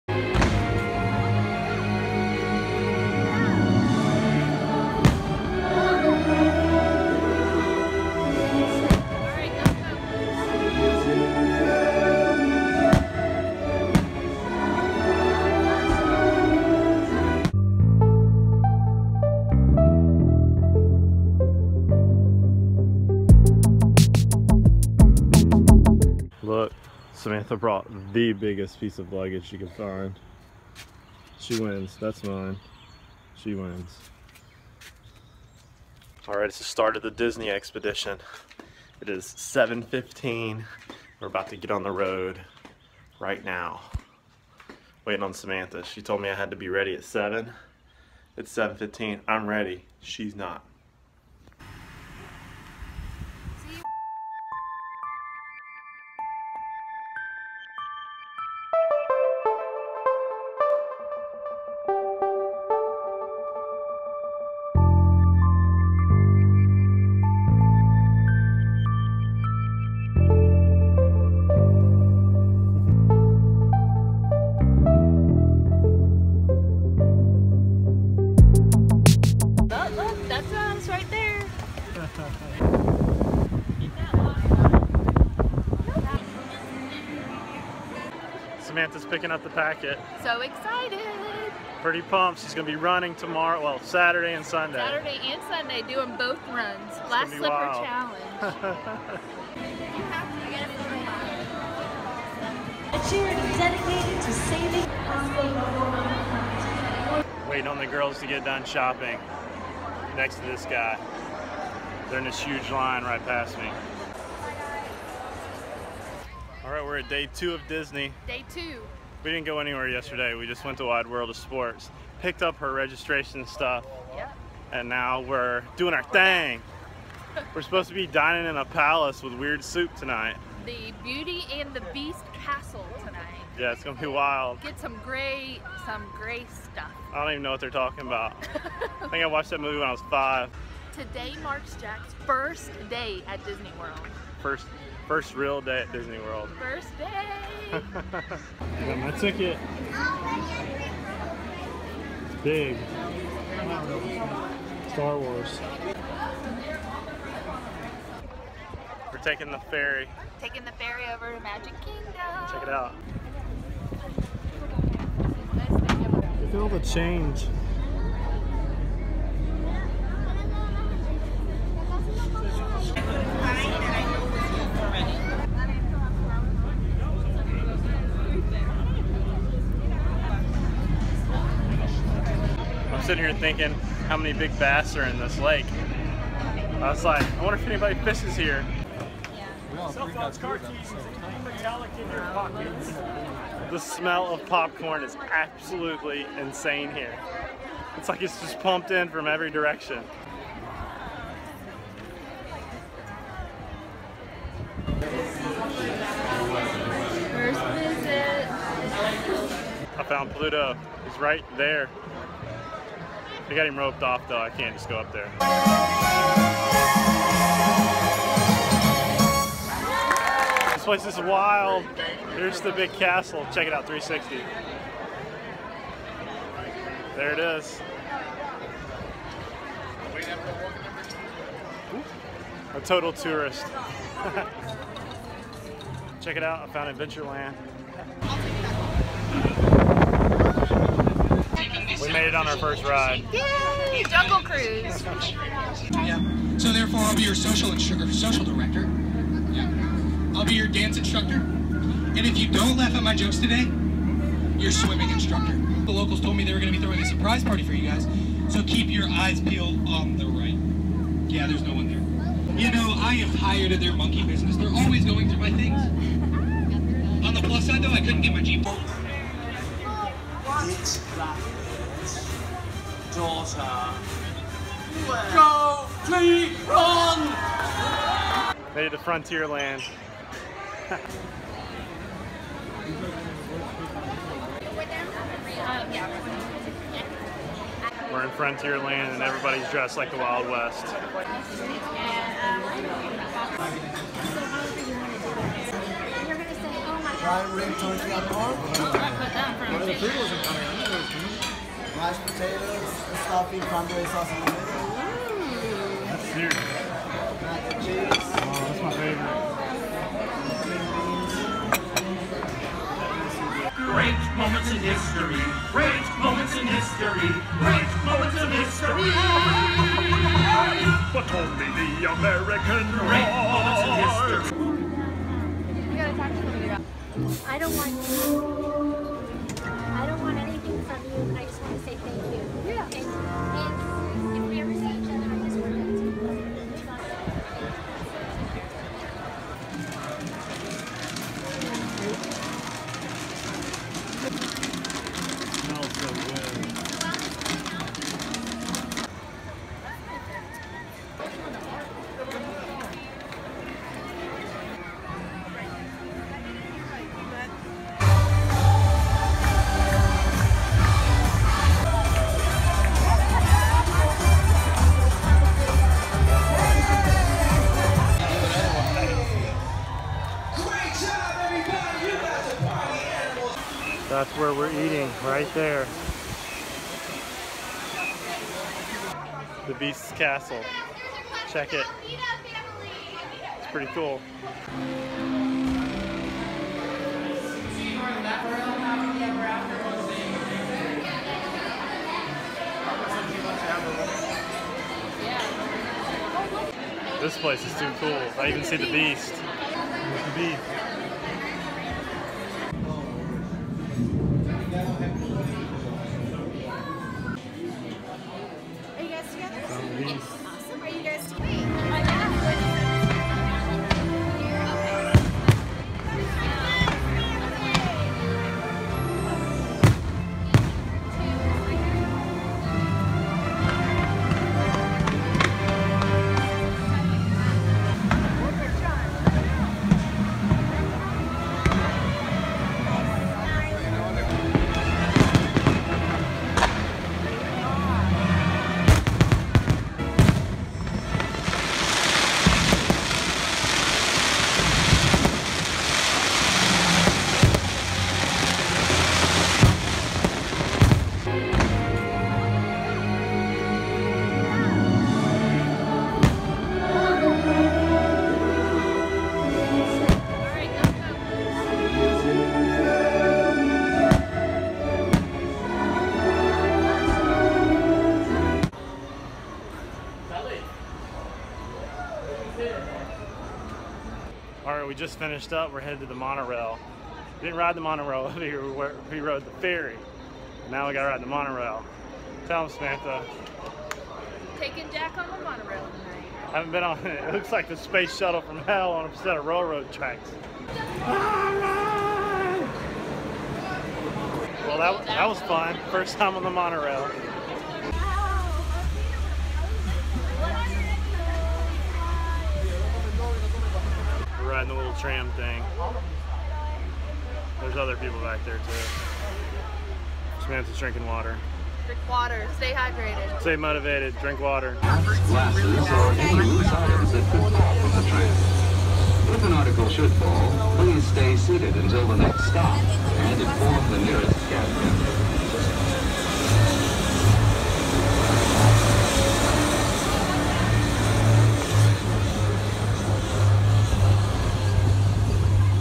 My FIRST Trip to DISNEY WORLD!
VLOG #291- In this VLOG, we took a family vacation to Disney World. This was my first time to Disney World. It was awesome . Samantha went to run the Princess 1/2 marathon run an 10k. The Run Disney crew did a great job organizing the event. If you have not been to Disney World yet you should definitely go "once!"